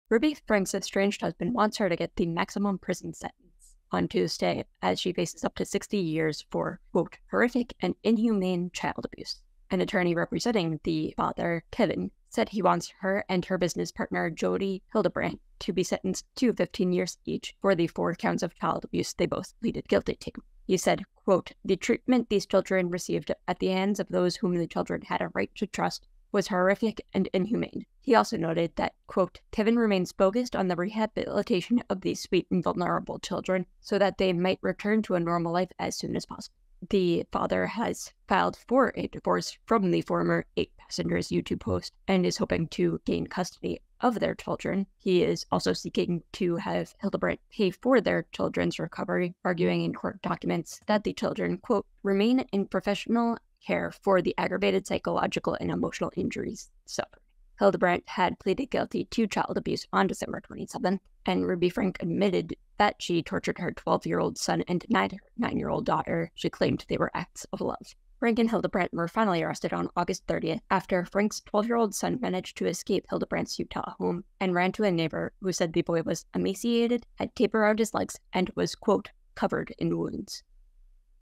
Ruby Franke’s husband wants vlogger to face max prison time for her child abuse
Ruby Franke’s estranged husband wants the disgraced mommy vlogger to get the maximum prison time Tuesday as she faces up to 60 years for “horrific and inhumane” child abuse. NY Post reporter Melissa Koenig shares this story.

Kevin Franke’s attorney relayed his wishes as his vlogger wife and her business partner, Jodi Hildebrandt, are sentenced Tuesday, having pleaded guilty to four counts of child abuse.

“We trust the judge to sentence them both to one to 15 years for each of the four counts, to run consecutively, and let the Utah State Board of Pardons decide if that should be shortened or other conditions imposed,” the husband’s attorney, Randy Kester, told Law & Crime.